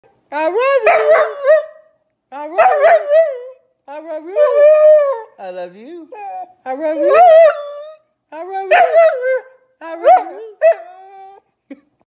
My dog says I love you
Boykin Spaniel Australian Shephard mix says I love you